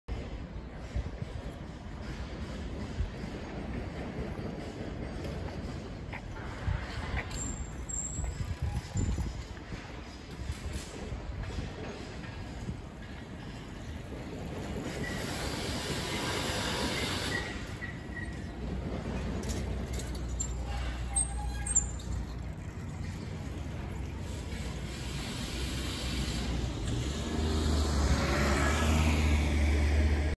押上踏切 桜橋通り 墨田区 2022年 3月31日 (木) 東武伊勢崎線 とうきょうスカイツリー駅付近 高架化工事
2022年 3月31日 (木)　IMG 0091
アイホン　iphone SE
東京スカイツリー　 2022
墨田区
Tokyo Skytree
Skytree
tokyoskytree　　東京スカイツリー
Tokyoskytree777.twitter.com